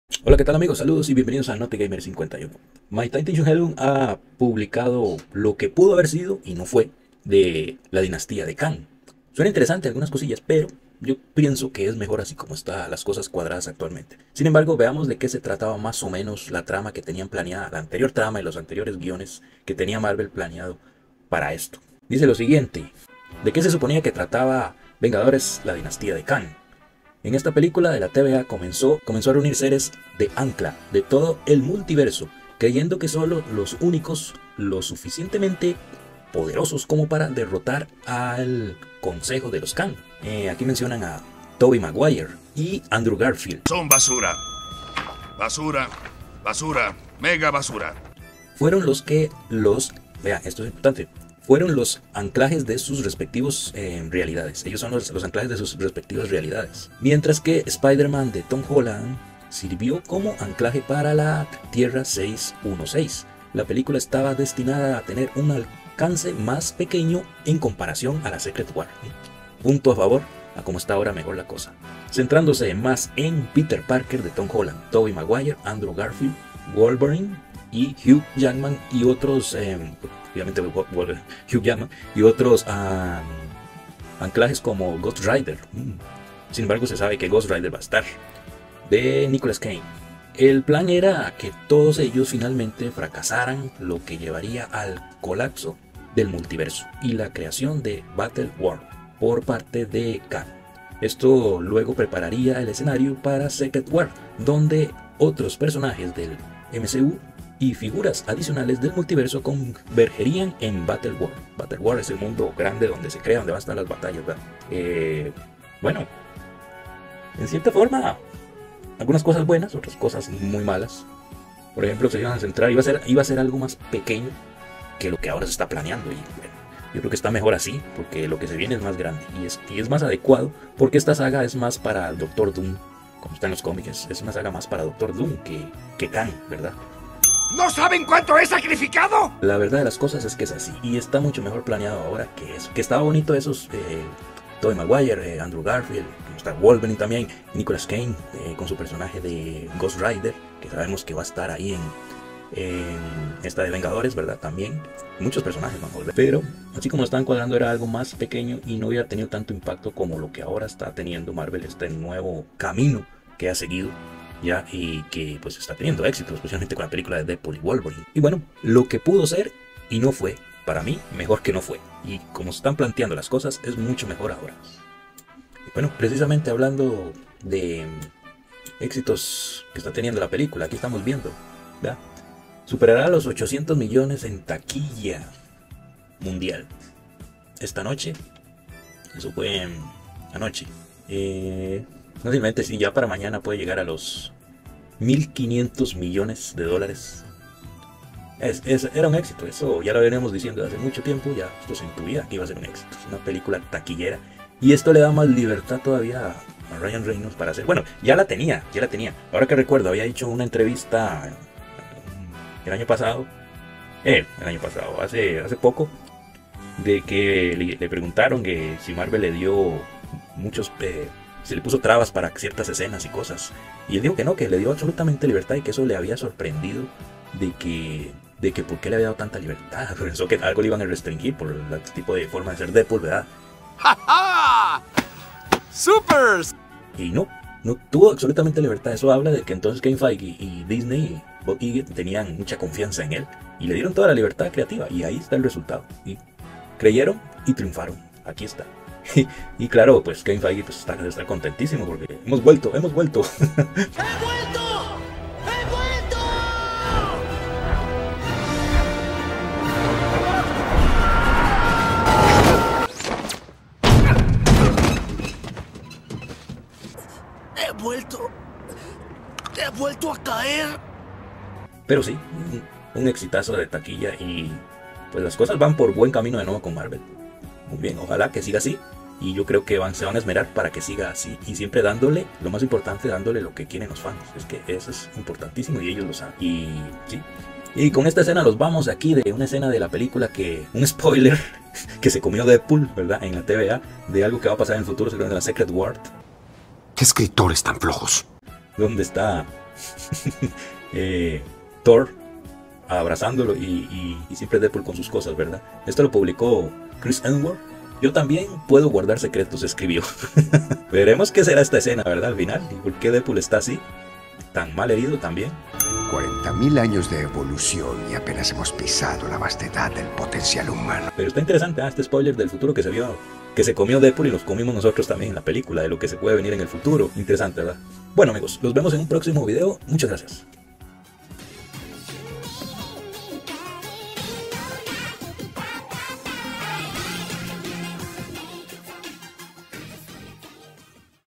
AVENGERS: THE KANG DYNASTY ¿De qué iba a tratar? Deadpool y Wolverine todo un éxito 🤑😎
En este canal se hablan de las siguientes cosas: Películas y series Marvel, figuras de acción ciencia ficción Transformers, Master of the universe, poquito más.

🎬 ✨👽 Quizás te interese: